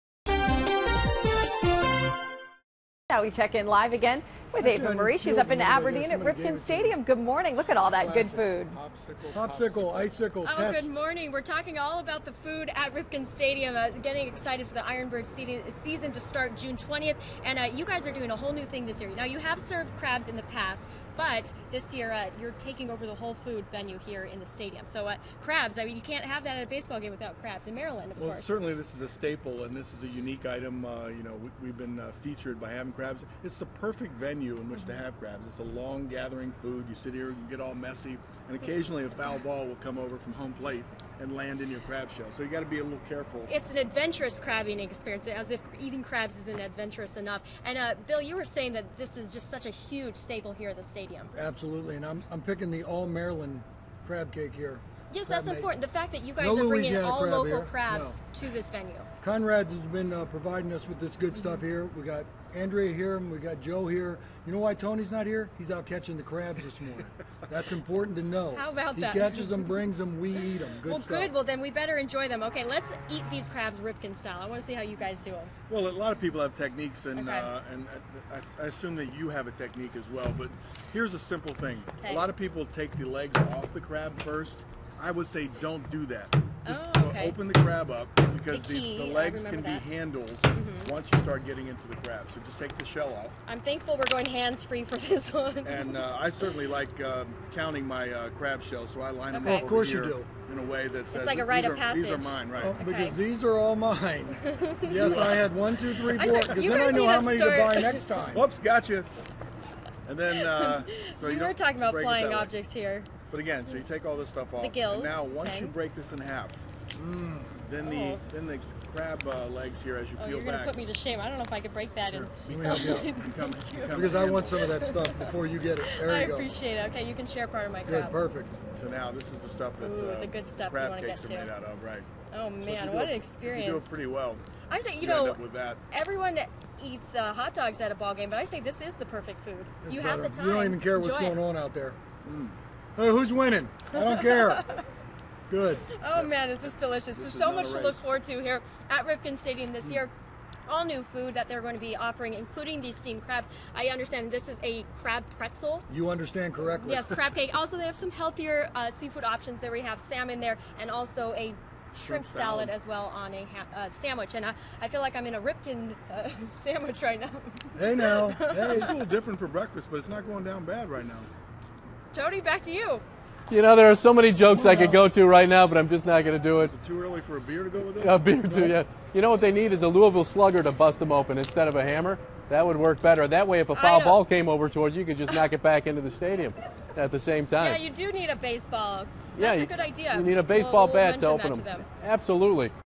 Ava eats crabs with Cal & Billy, talks Ironbird food
Ava Marie visits Ripken Stadium in Aberdeen, where she talks about the all-new food items available at Ironbirds games this year.